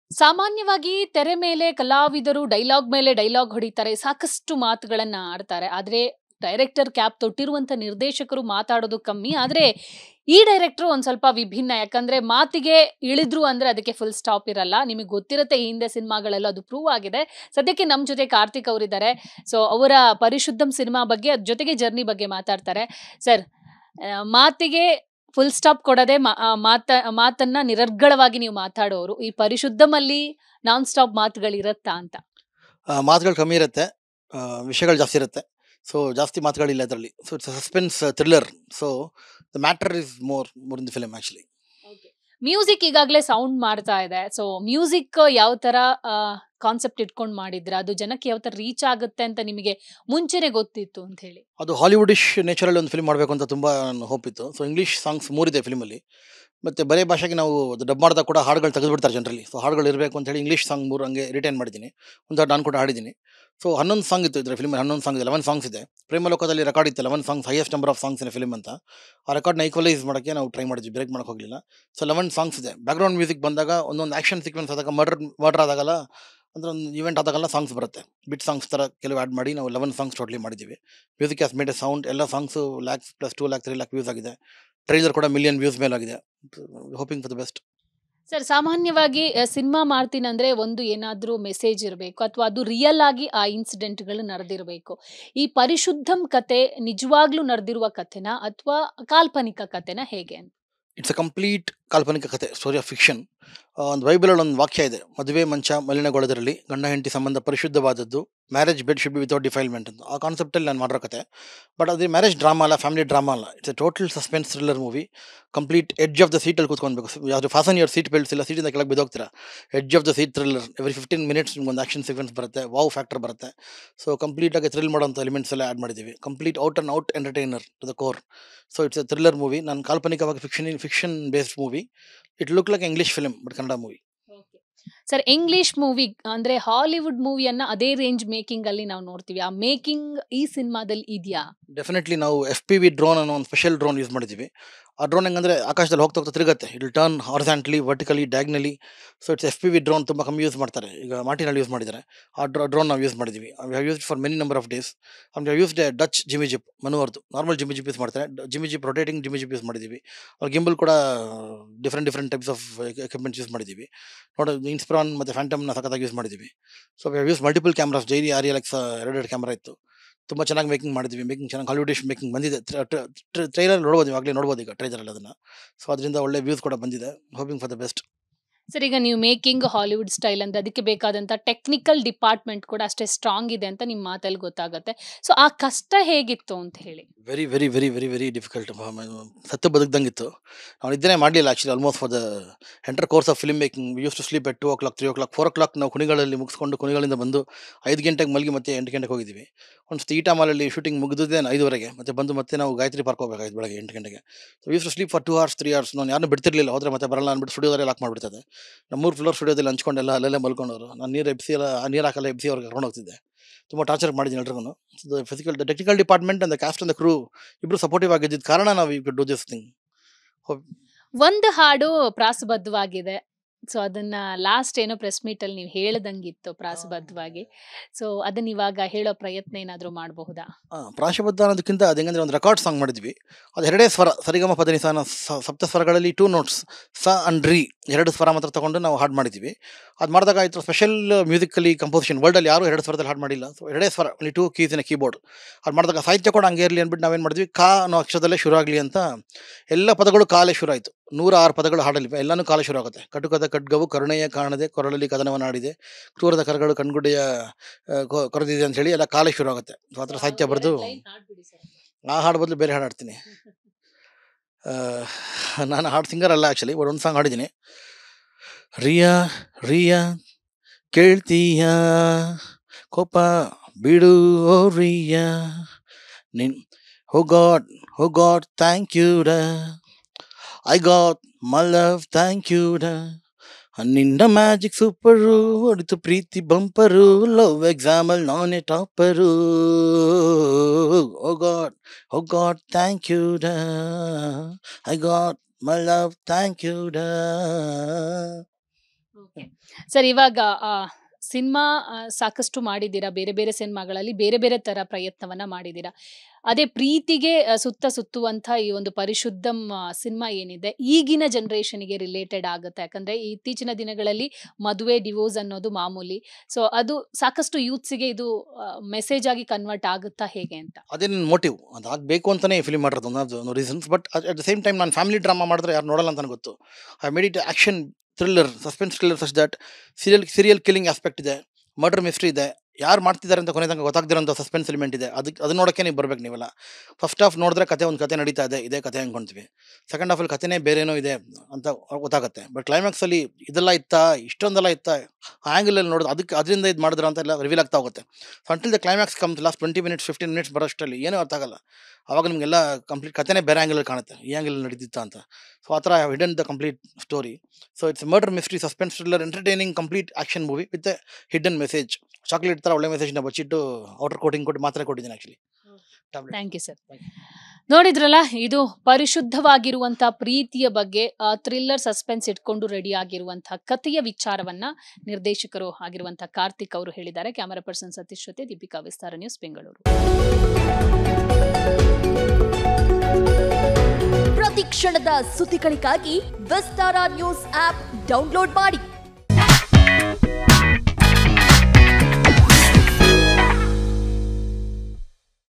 ಈ ಸಿನಿಮಾ ಸೀಟ್‌ ತುದಿಯಲ್ಲಿ ಕುಳಿತು ನೋಡಬೇಕಂತೆ! | Director Karthik Venkatesh Talks About Parishudham Film
ಇದೇ ತಿಂಗಳು 27ಕ್ಕೆ ಬಿಡುಗಡೆಯಾಗುತ್ತಿರುವ ಪರಿಶುದ್ಧಂ ಸಿನಿಮಾ ಬಗ್ಗೆ ಅದರ ನಿರ್ದೇಶಕ ಆರೋನ್‌ ಕಾರ್ತಿಕ್‌ ವೆಂಕಟೇಶ್‌ ಅವರು ವಿಸ್ತಾರ ನ್ಯೂಸ್‌ ಜೊತೆ ಮಾತನಾಡಿದ್ದಾರೆ.

ಈ ಸಿನಿಮಾ ಸೀಟ್‌ ತುದಿಯಲ್ಲಿ ಕುಳಿತು ನೋಡಬೇಕಂತೆ! | Director Karthik Venkatesh Talks About Parishudham Film | Vistara Cinema